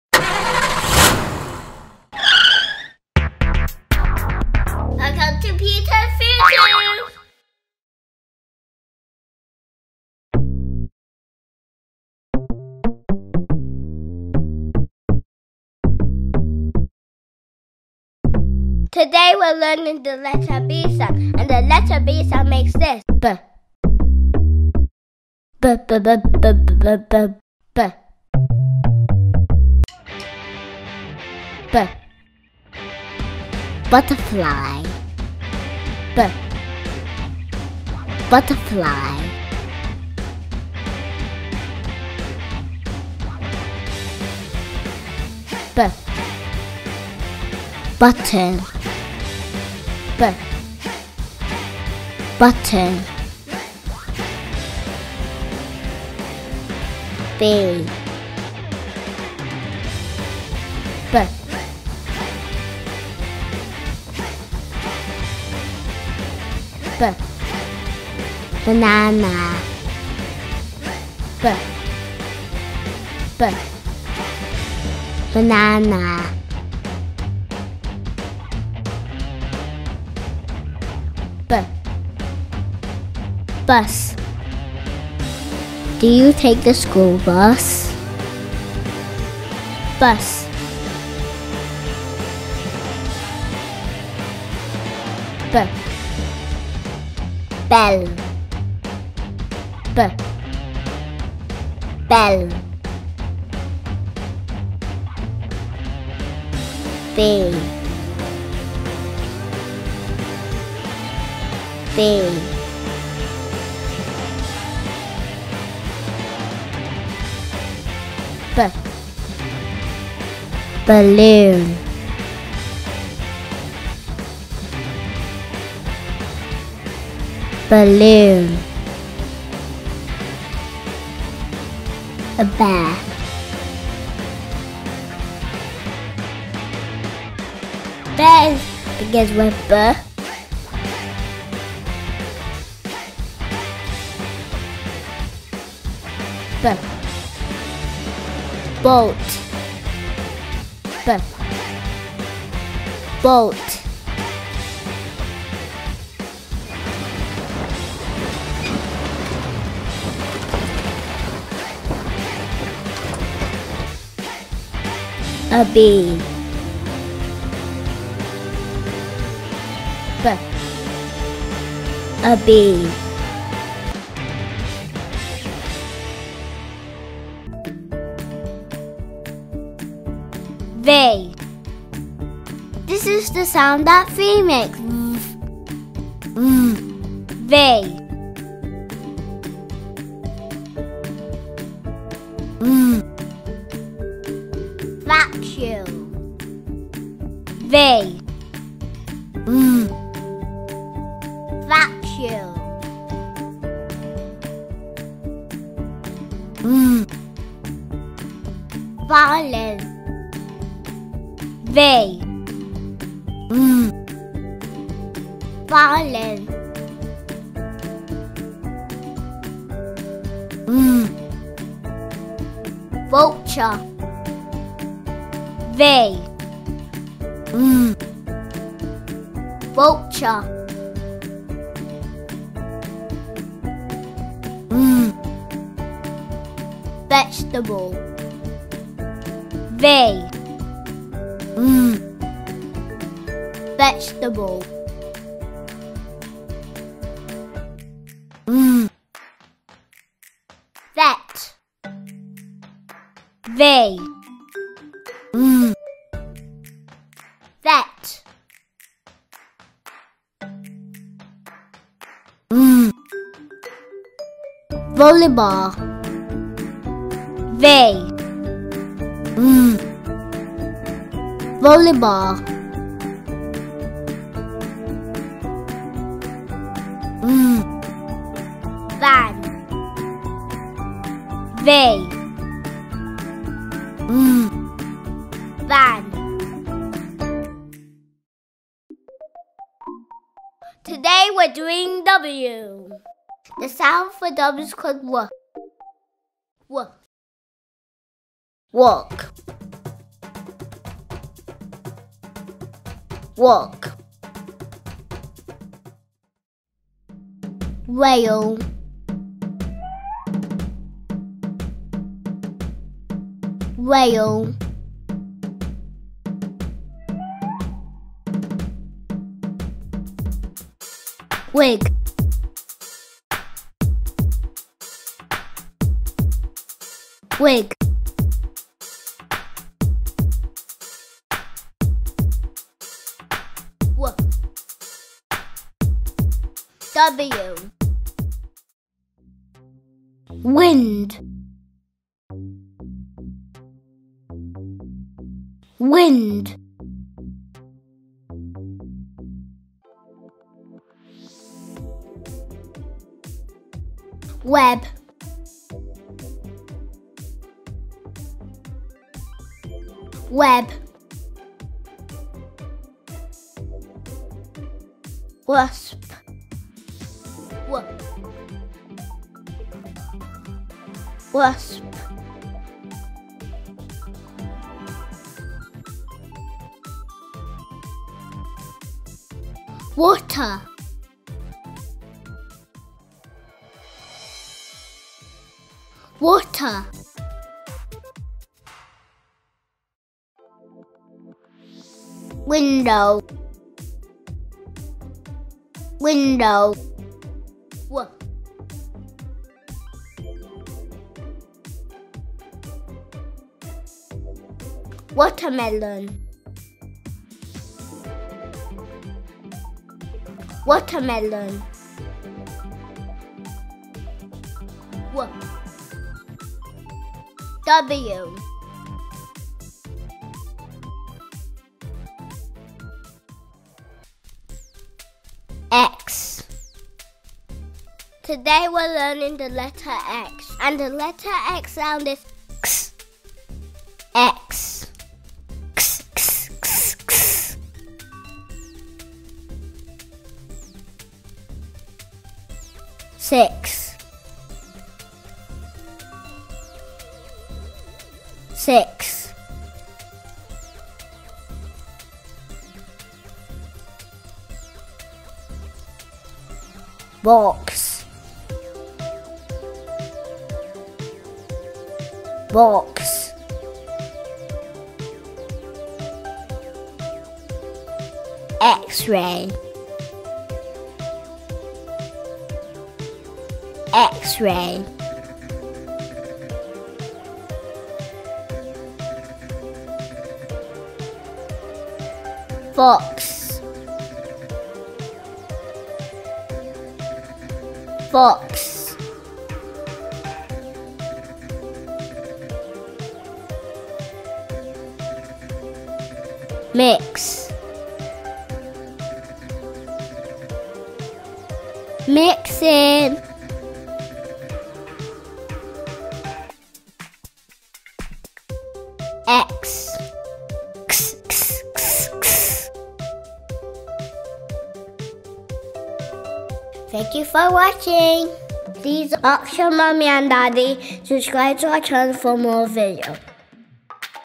Early Learning | ABC Phonics | Alphabets Compilation Song for Children | Kids Phonics for preschool
Welcome to Peterphil Toon.

Let's learn phonics together! This is a phonics compilation video.
Our goal is to help make learning fun and enjoyable by creating educational lyrics and attractive animation for children.

Sing along, dance to it and play along.